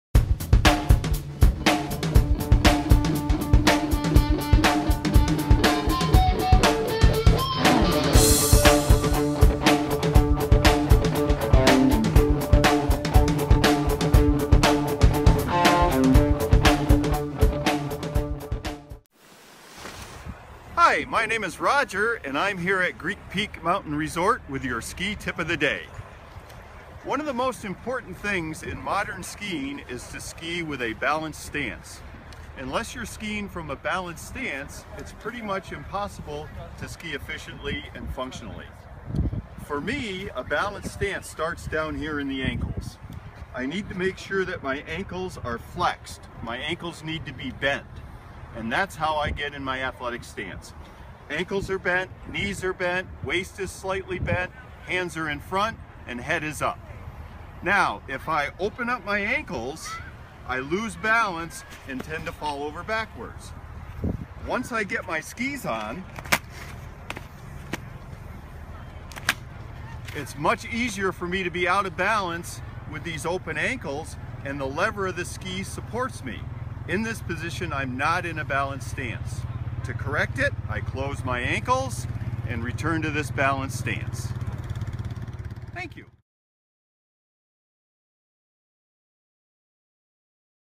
Greek Peak Ski Tip of the Day - Balanced Stance and Closed Ankles
Roger Zilliox, a veteran ski instructor here at Greek Peak Mountain Resort, demonstrates the importance of a balanced stance and closed ankles.

His resume includes:
PSIA Level 3 Alpine, USSA Level 1 Coach
Night Supervisor & Member Of Our Training Staff